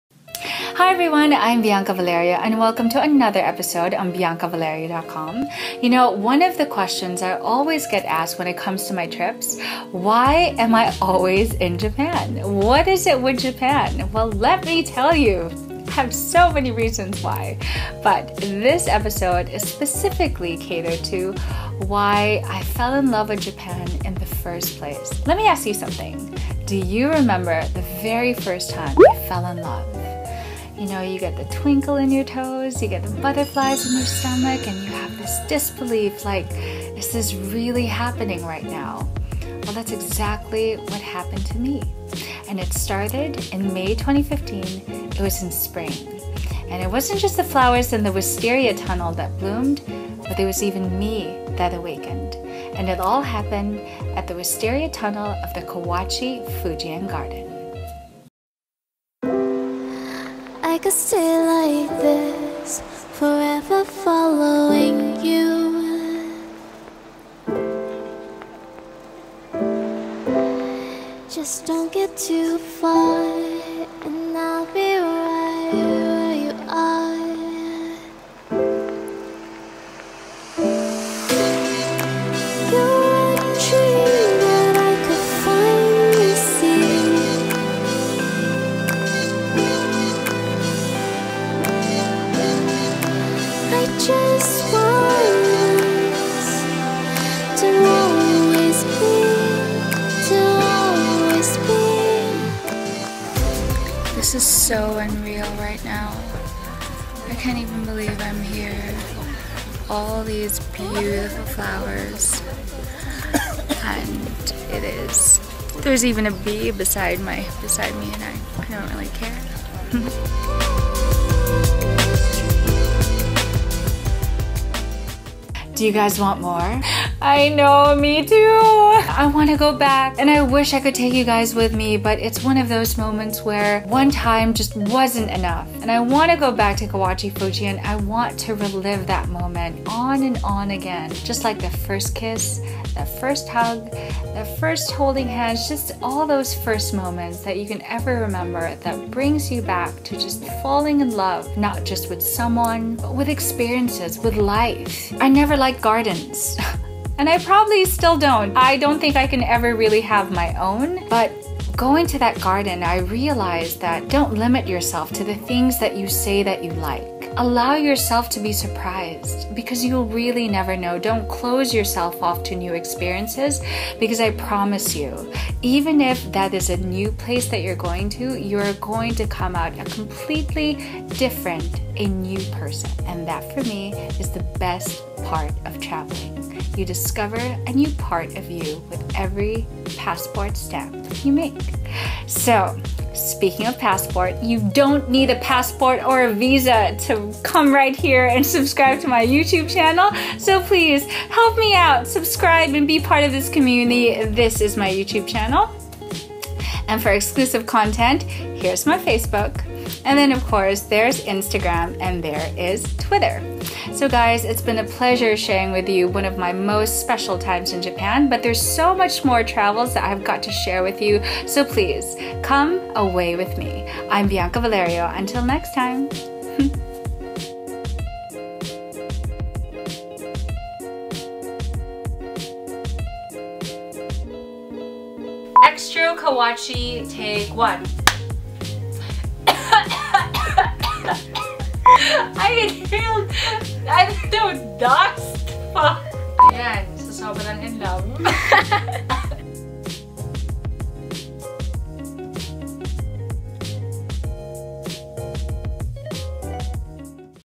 Kawachi Fujien Garden | Wisteria Tunnel | JAPAN | Bianca Valerio
Model-Host-Author, Bianca Valerio finds herself falling in love in the most unlikely of places: the magical Kawachi Fujien Wisteria Tunnel in Fukuoka, Japan.

Check out Bianca's travel photos and read the full story on this episode